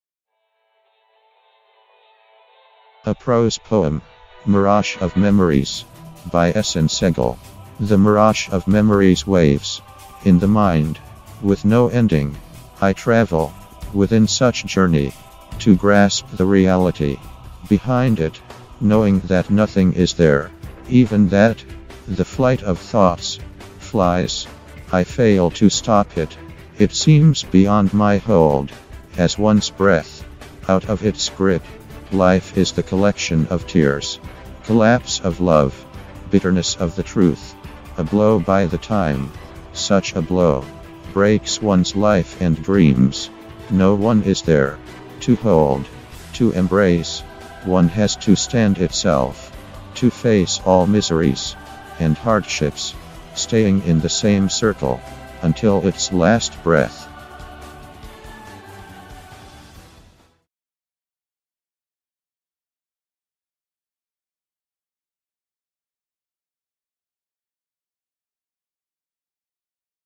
Ehsan Sehgal Prose Poem-Mirage Of Memories, with background music
A prose poem, Mirage Of Memories, with background music, by famous poet Ehsan Sehgal